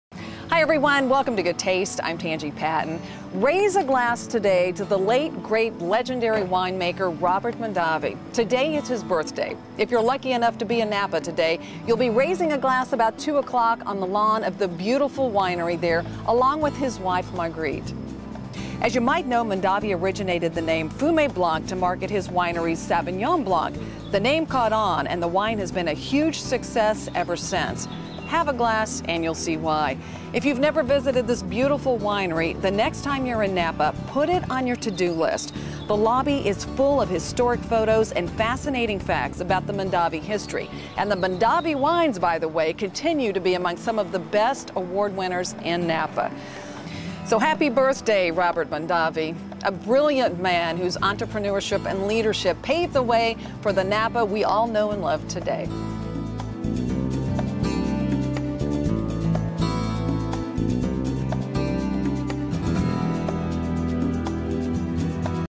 jun09_robert_mondavi.mov
Raise a glass today to the late wine great wine entreprenuer Robert Mondavi. Today is his birthday.

If you're lucky enough to be in Napa at the legendary winery that bears his name, you'd be raising a glass in his honor this afternoon.  His wife Margrit will be at the winery to toast her husband with a glass of Fume Blanc.

As you might know Mondavi originated the name Fume Blanc to market his winery's sauvignon blanc -- the name caught on and the wine has been a huge success ever since.  Have a glass, and you'll see why.

If you've never visited this beautiful winery, the next time you're in Napa put it on your to do list. The lobby is full of historic photos and fascinating facts about the Mondavi history.

So here's to the late Robert Mondavi...a brilliant man whose passion for wine paved the way for the incredible success of Napa and its wines.

If you'd like to send your thoughts along to the winery, you can do so